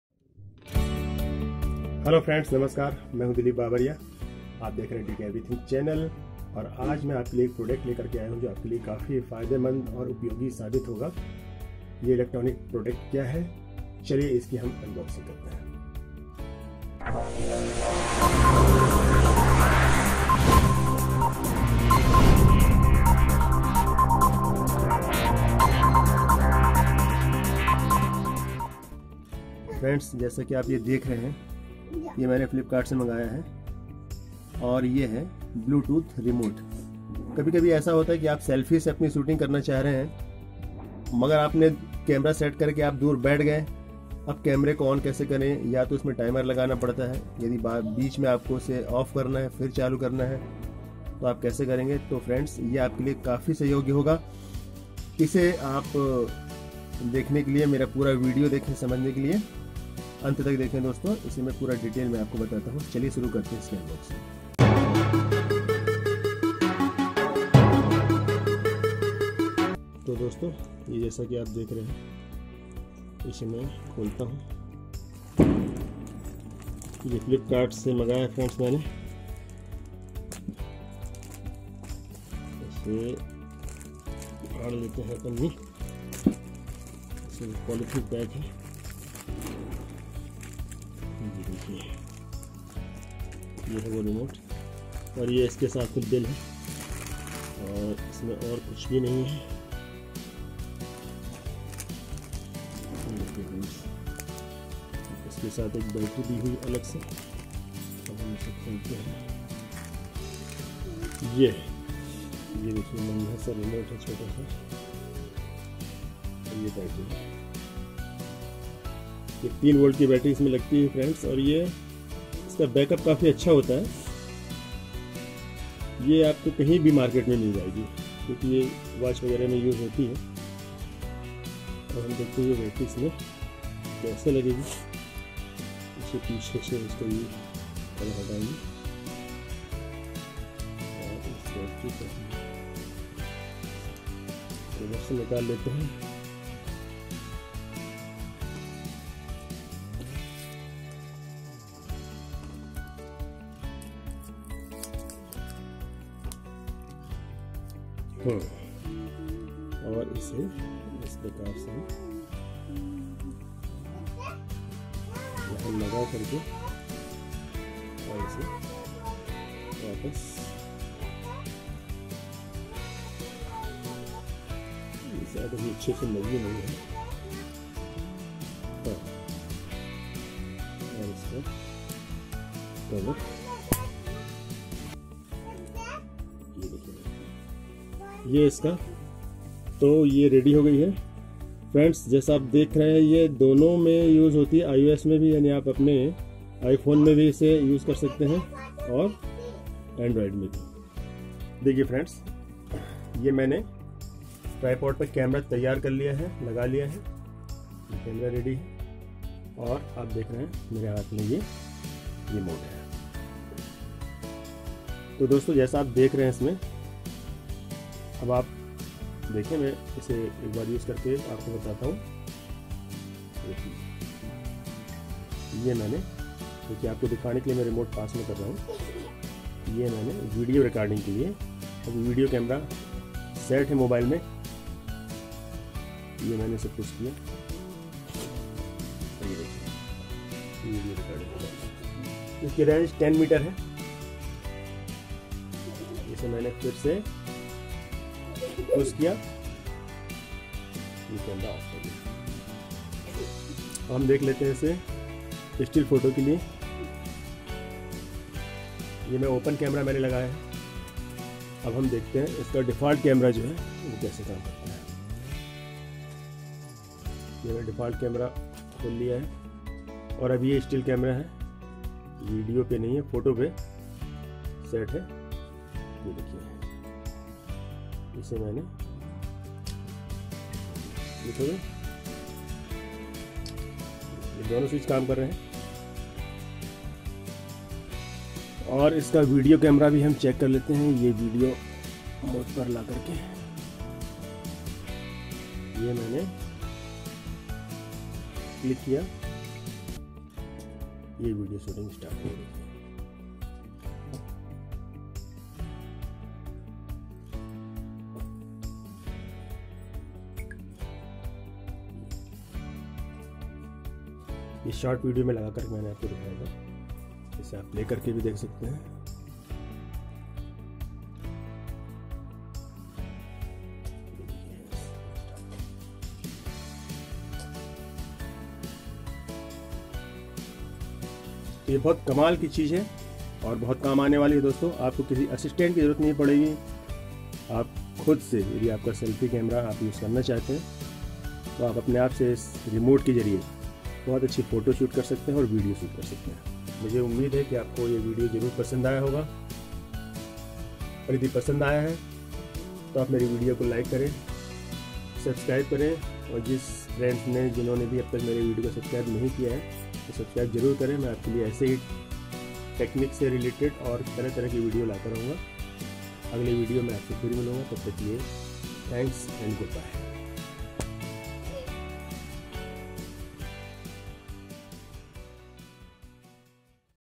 Bluetooth Remote Shutter for Android - BY DK EVERYTHING
दोस्तों अब आपको सेल्फी कैमरा use करने के लिए किसी पर डिपेन्ड होने की जरुरत नहीं पड़ेगी ये सेल्फी remote के इस्तेमाल से